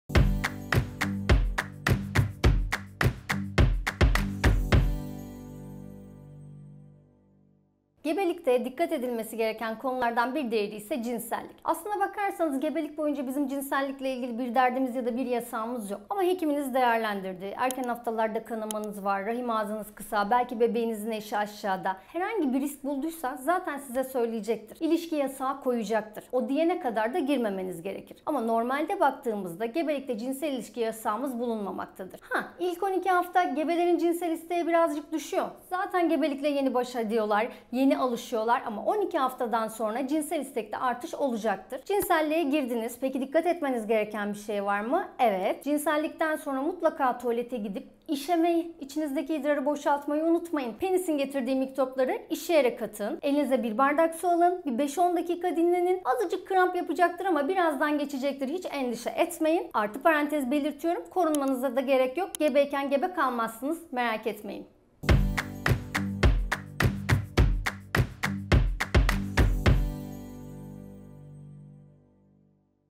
Gebelikte Cinsel İlişki Olur mu? Hamilelikte Cinsel İlişki Tehlikeli Mi? - Mavi Kadın
Gebelikte cinsel ilişkiye dair çiftlerin kafasında birçok soru olabiliyor. Bebeğe zarar gelir korkusuyla gebelikte cinsel ilişkiden çekinenlerin sayısı da oldukça fazla. Gebelikte cinsel ilişki gerçekten zararlı mıdır? Bebeğe zarar verir mi? Gebeler ne zaman cinsel ilişkiye girmemeli? Doç.Dr. Pınar Kadiroğulları anlattı.